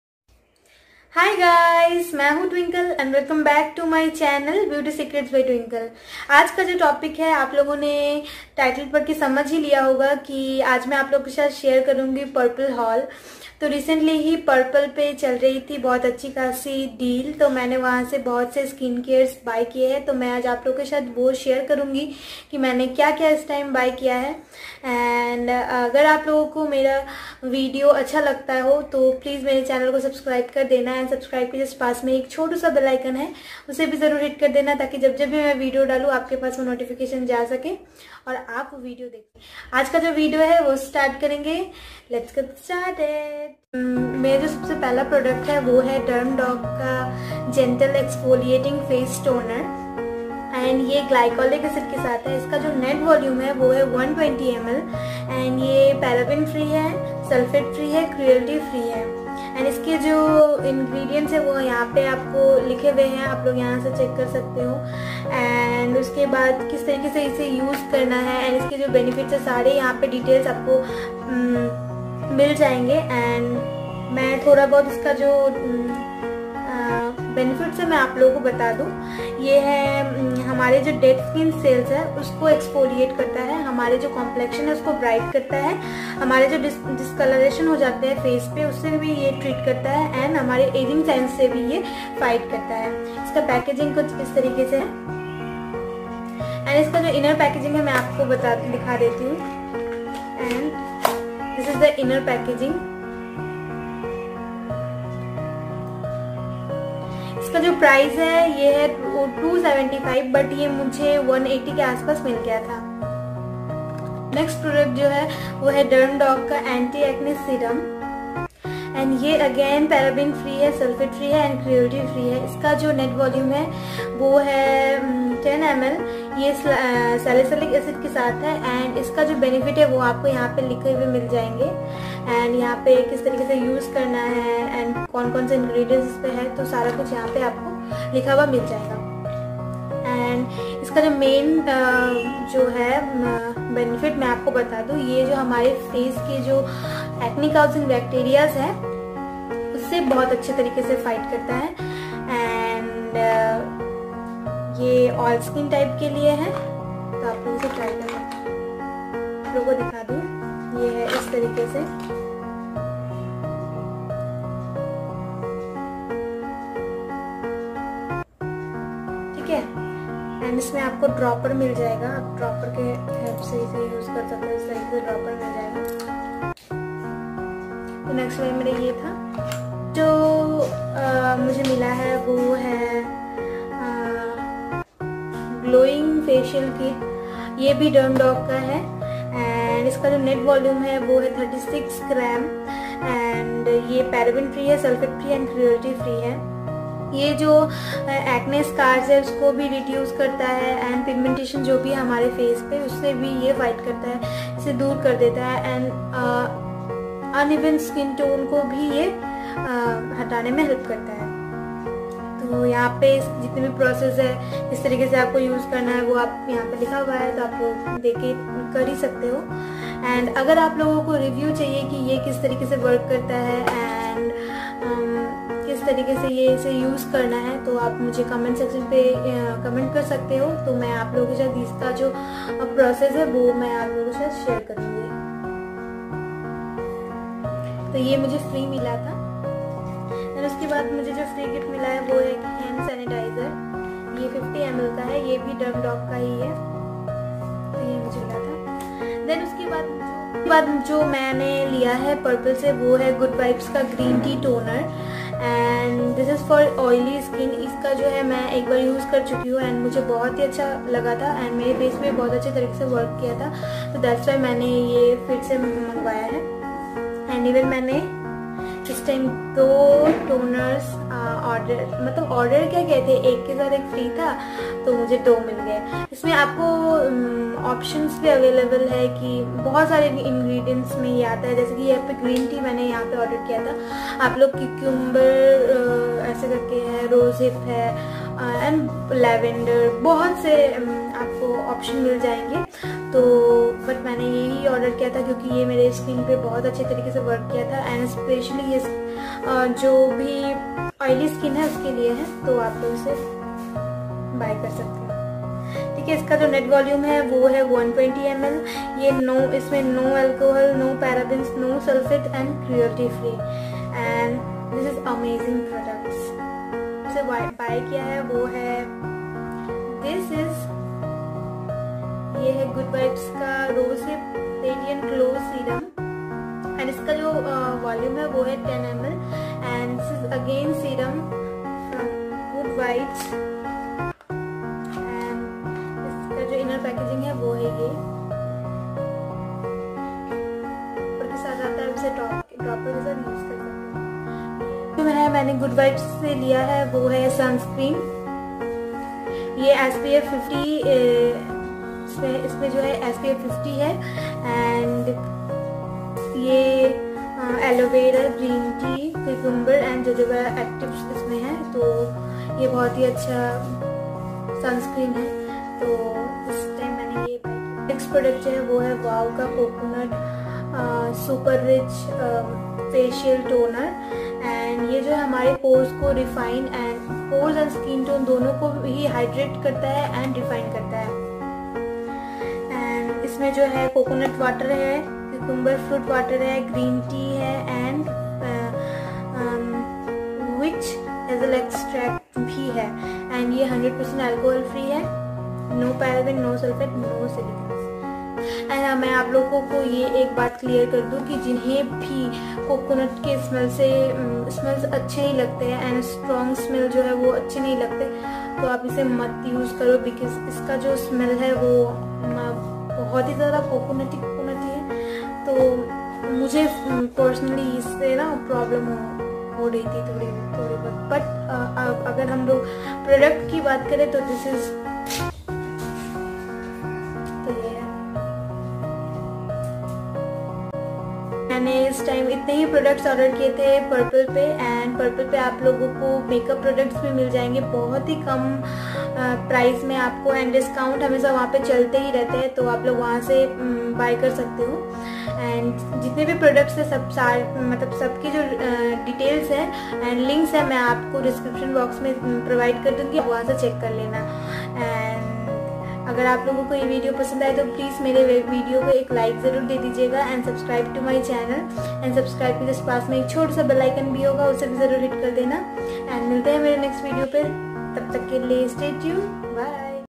Purplle houl****||beauty secrets by twinkle
Hiii gyzzz I m sharing a tini mini purplle houl..I hope u will like it. ❤

Product details:-

DermDoc Gentle Exfoliating Toner with Glycolic Acid (120 ml)

DermDoc Anti Acne Face Serum with Salicylic Acid (10 ml)

DermDoc Skin Glowing Facial Kit with Arbutin (36 g)

Good Vibes Toner - Green Tea (120 ml)

Good Vibes Radiant Glow Face Serum - Rose Hip (10 ml)

Good Vibes Wide Spectrum Sunscreen with SPF 50 (50 g)

WOW Skin Science Coconut Super Rich Facial Toner (200 ml)